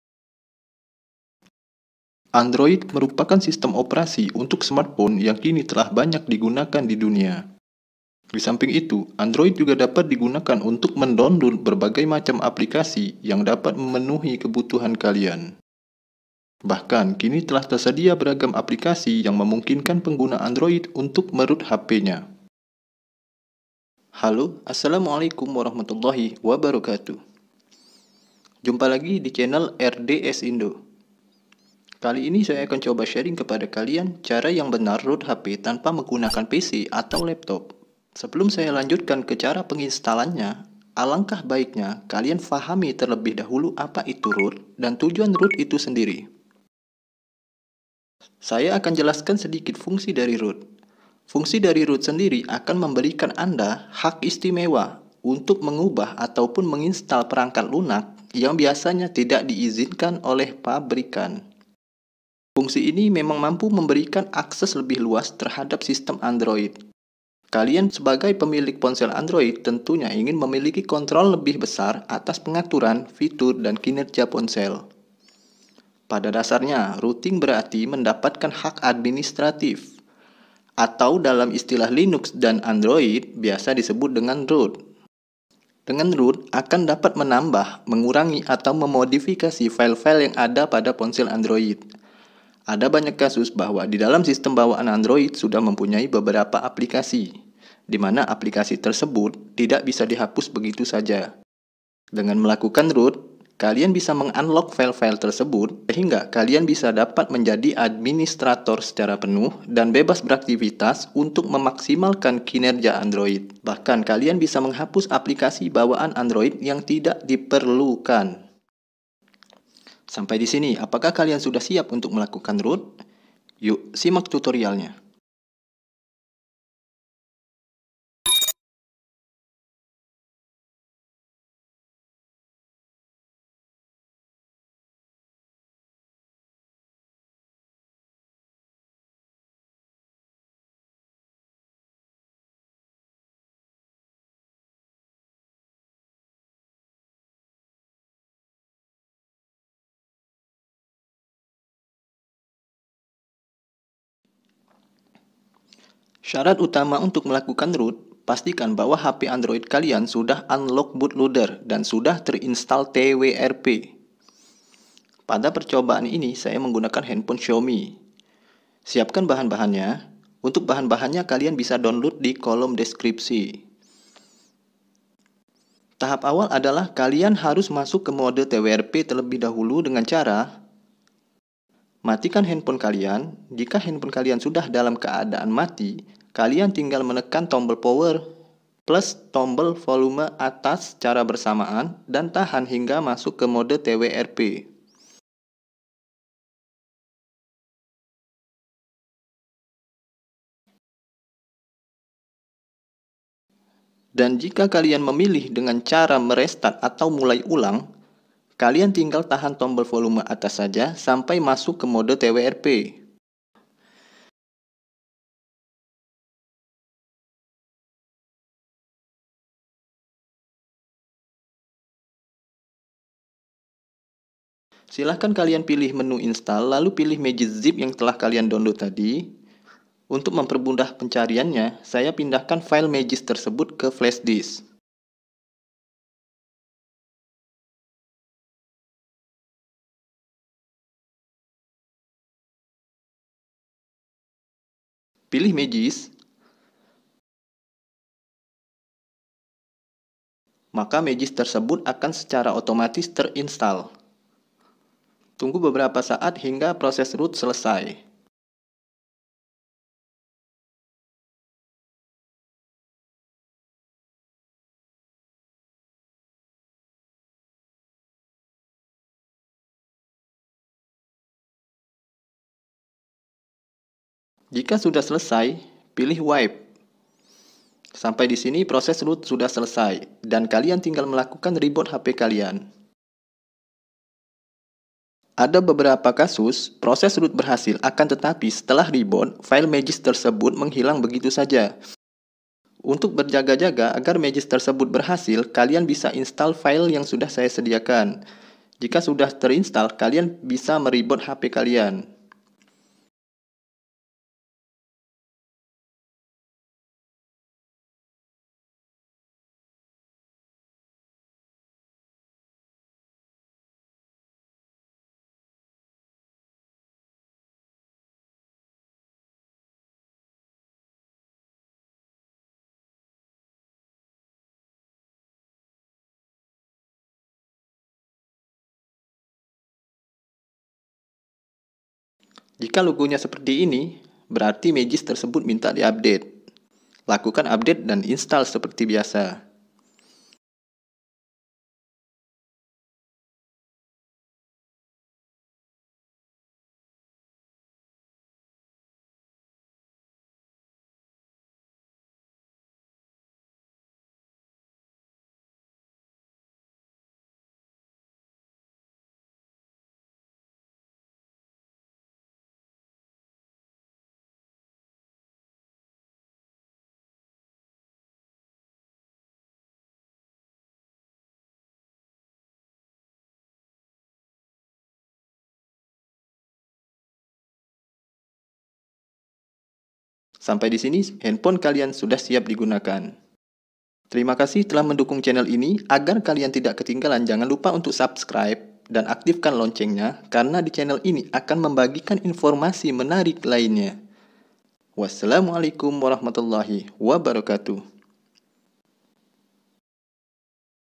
Cara Benar Root Hp ANDROID Tanpa Menggunakan PC | how to root android phone without pc
Cara yang Benar Root Hp ANDROID Tanpa Menggunakan PC atau Laptop
how to root android phone without pc

Cara ini berlaku untuk semua tipe HP Android